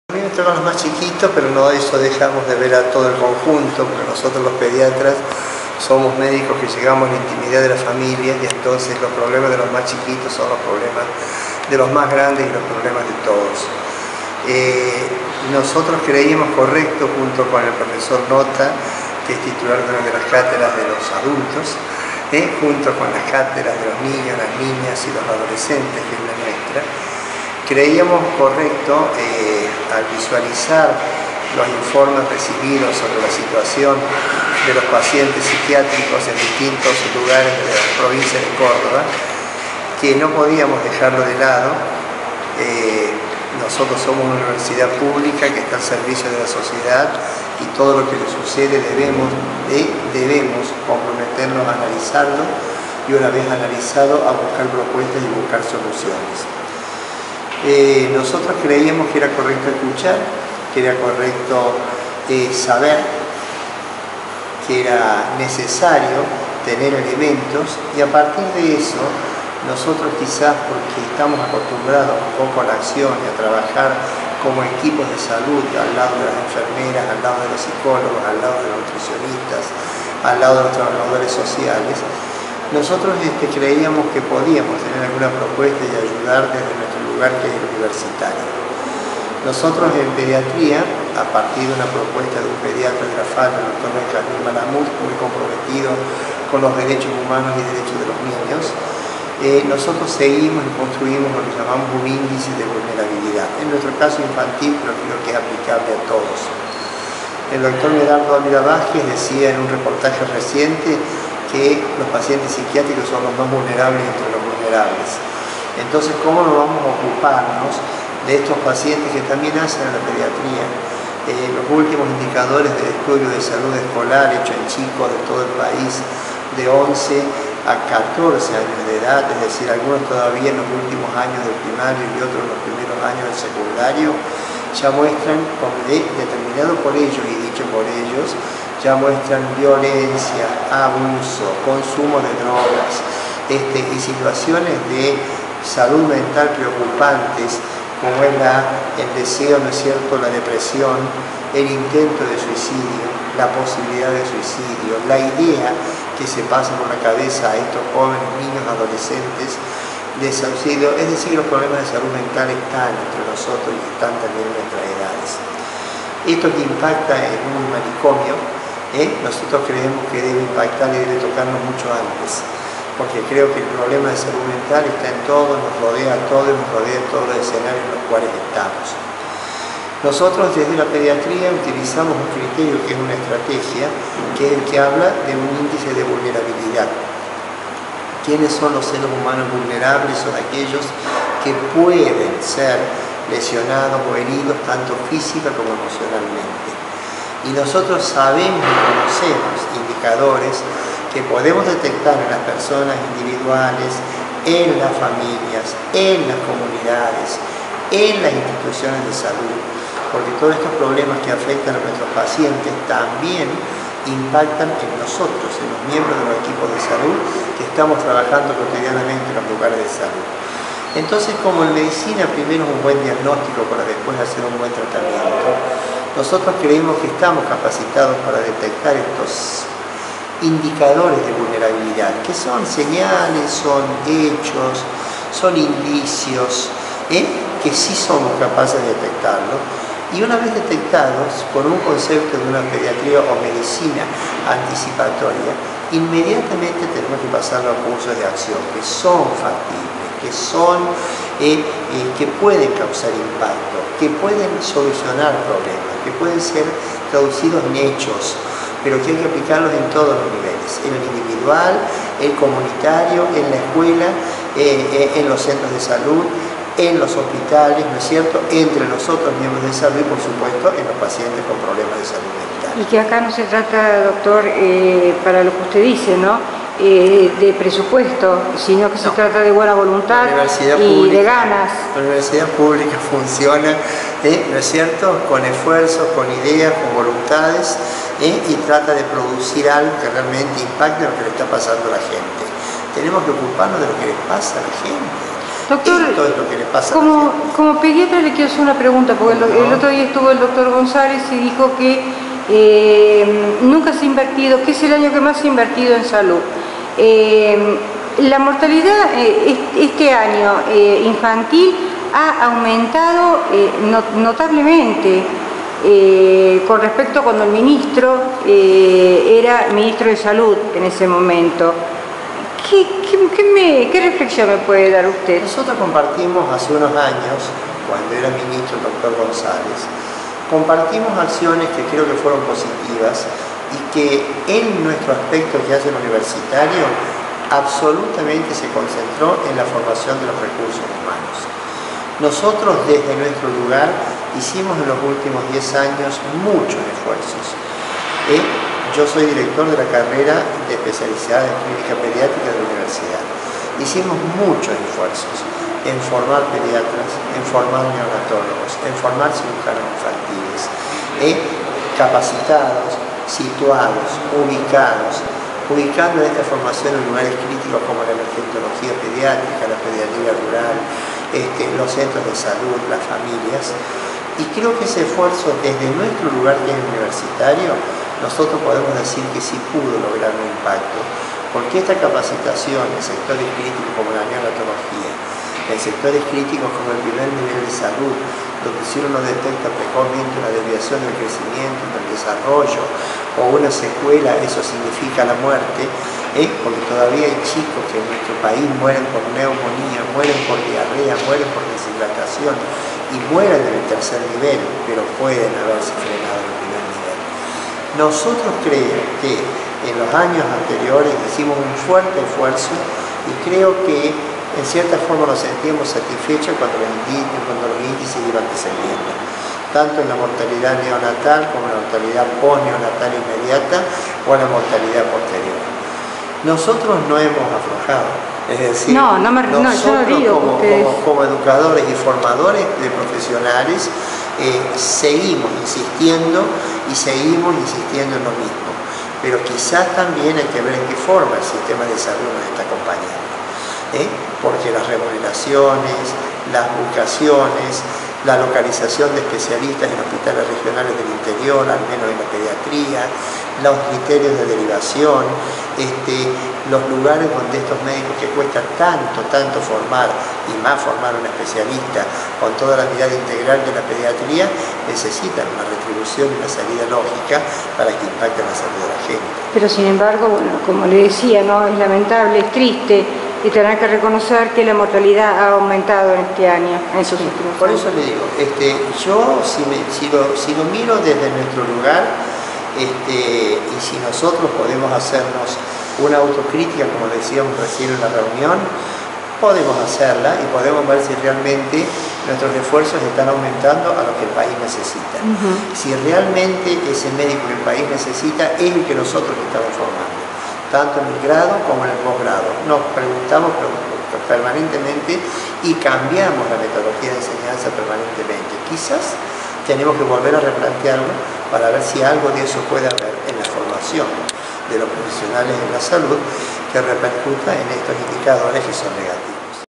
DR. DANIEL QUIROGA - Jefe de Cátedra de Cínica Pediátrica de la UNC
El Dr. Daniel Quiroga, Jefe de Cátedra de Clínica Pediátrica de la UNC, en diálogo con informabellville, afirmó que creían necesario asistir a esta jornada sobre salud realizada en el Hospital Clínicas, ya que los pediatras llegamos a todos los hogares por ocuparnos de los más chicos. "Analizando la situación de los pacientes psiquiátricos, como integrantes de la sociedad debemos ver qué podemos hacer para solucionar estos problemas. Desde nuestro lugar universitario en pediatría, hemos detectado un índice de vulnerabilidad, que son los que pueden ser heridos tanto física como emocionalmente.  . Los últimos inicadores muestran que en los últimos años del primario y comenzando el secundario hay severas muestras de conductas erróneas, o sea que el problema de salud mental está instalado también aquí. Chicos que quieren suicidarse, se drogan, entre otras cosas. Todos estos problemas que afectan a la sociedad impactan en nosotros.